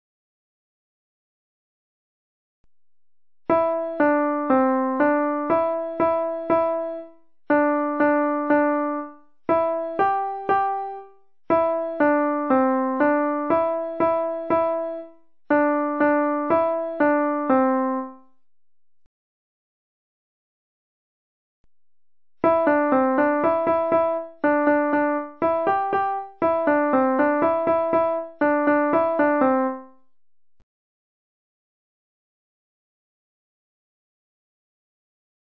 Quavers and eighteen notes: the key
Watch how english folk song is displayed in quavers or eighteen notes. Learn how to sing with My first Ebook of music.